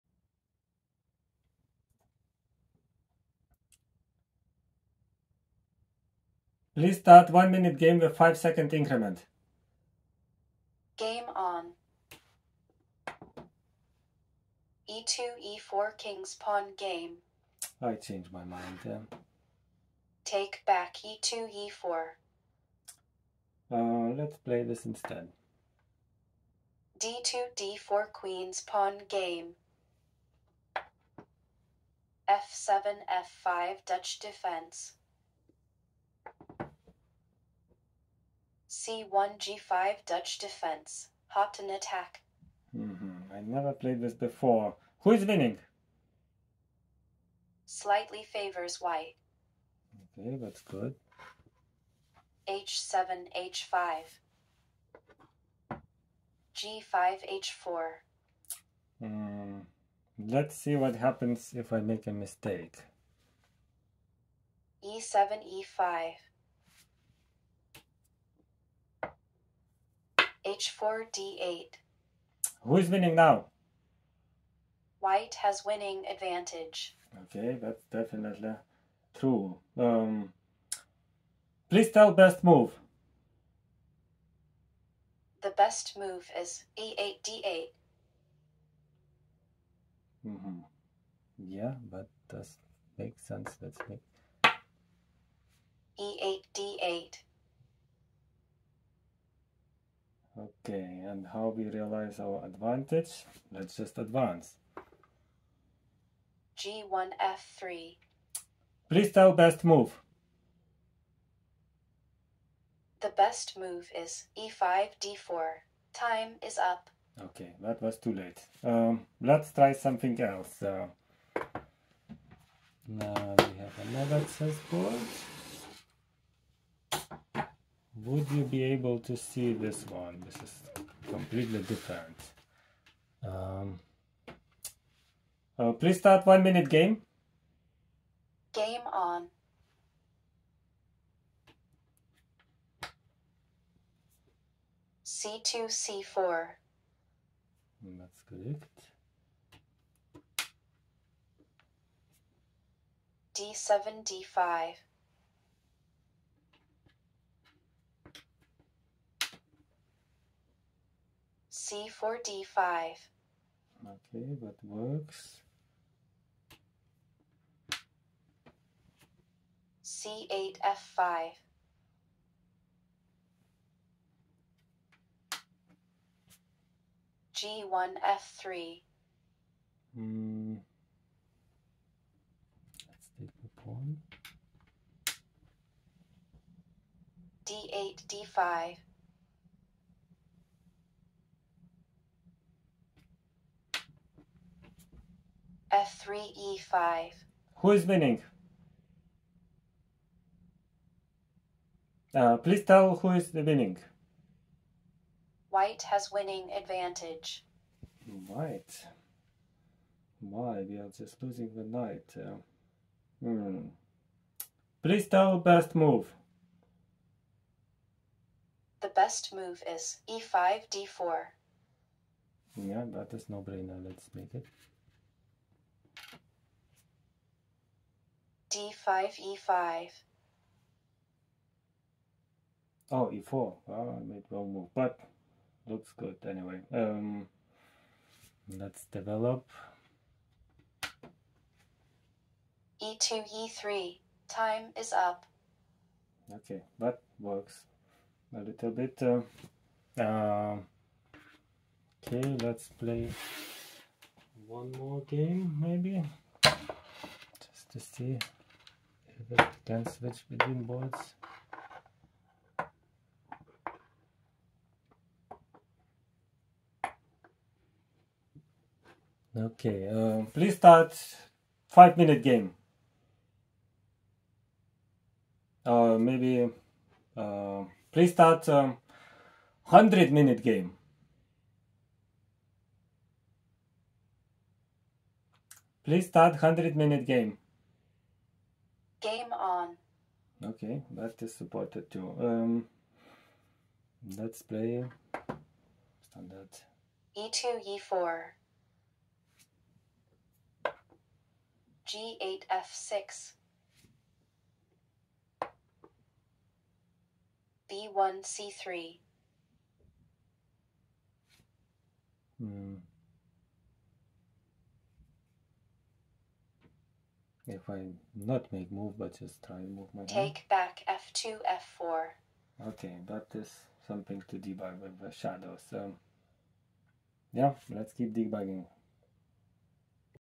Hands Free Chess Clock: 2 very different boards
Demonstration of NVIDIA Jetson Orin Nano Developer Kit powered hands free chess clock. Clock is started by a voice command. Camera automatically detects when move is made allowing to stop currently active clock and start opposite color clock. Software has build in chess engine to detect checkmate, stalemate and other game end conditions.